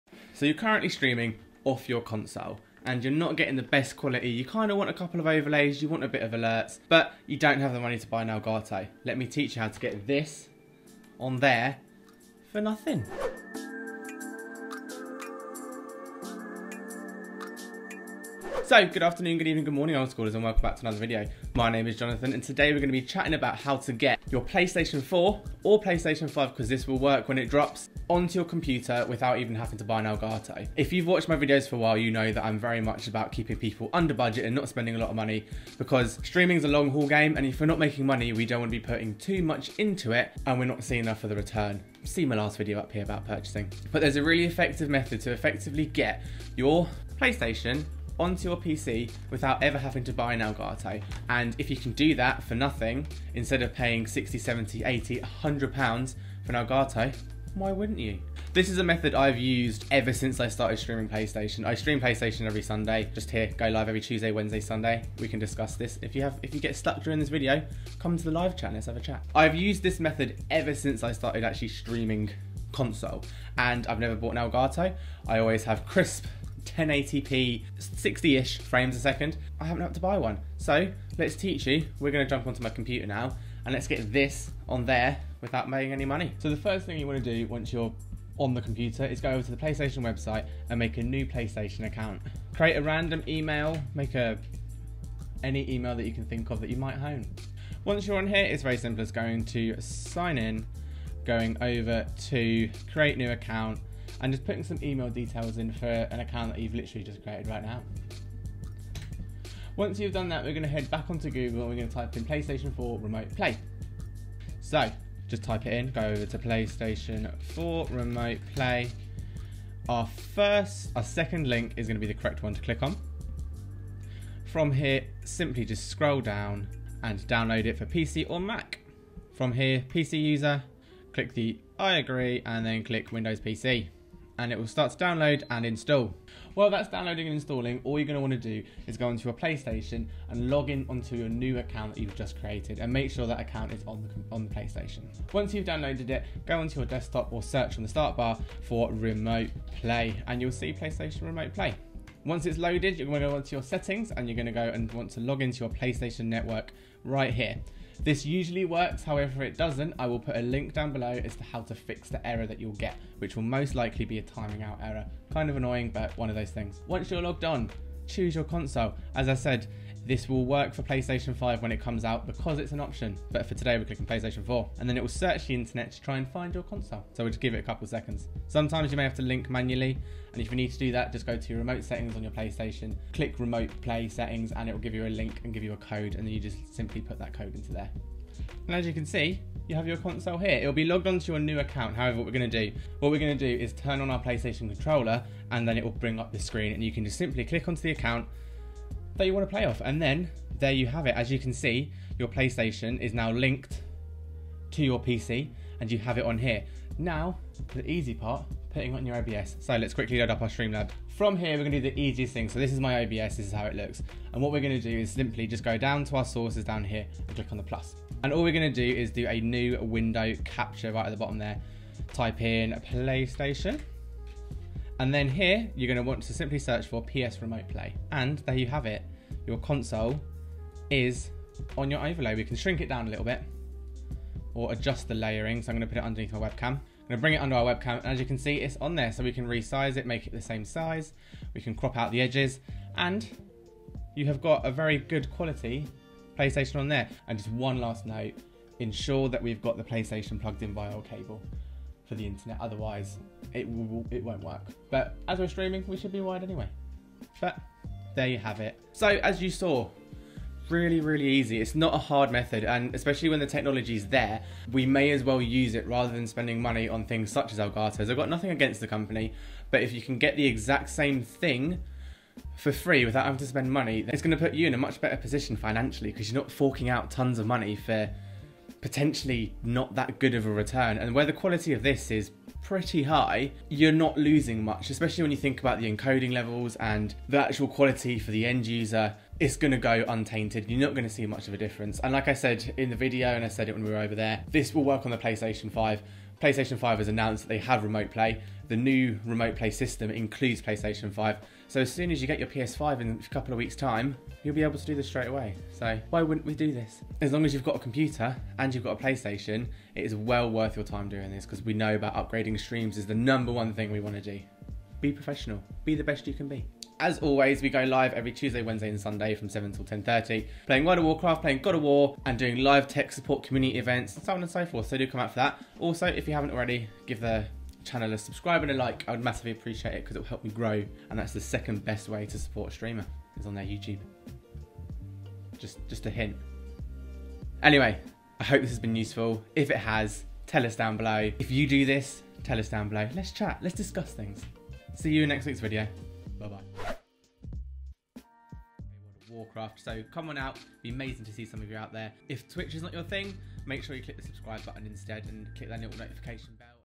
How to Stream From your PS4 or PS5 To PC Free! 2022
In today's video we talk about How to Stream From your PS4 or PS5 To PC Free! Yes, I did say that we could do this to the PS5 because we can! Honest! The newest update of Remote Play includes remote play options for PS5 so this will work the minute the console drops. You know I love a good bit of money saving so, please do follow these tips to stream to your ps4 to pc without ever needing a elgato capture card!

All music from Epidemic Sounds